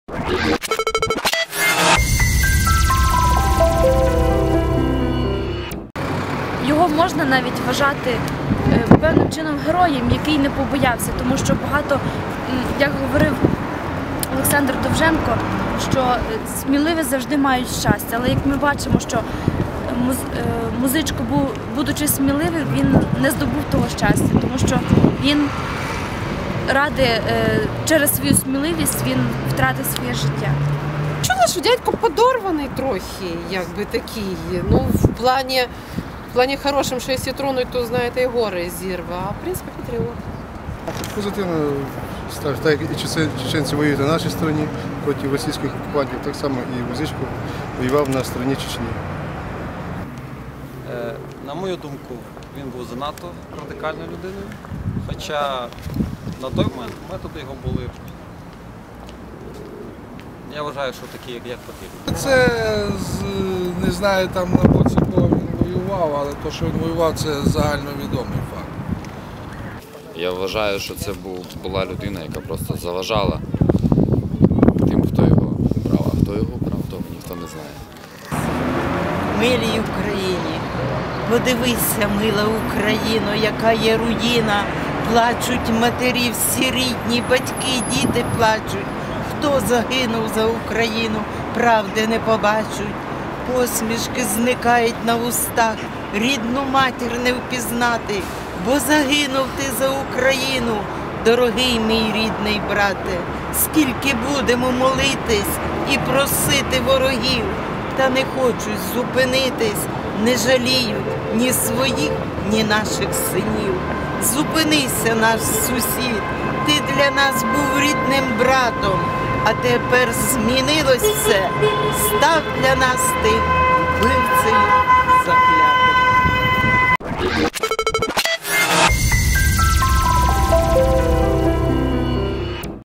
Рік тому вбили Олександра Музичка. Як рівняни згадують українського Робін Гуда
Репортаж до річниці смерті Олександра Музичко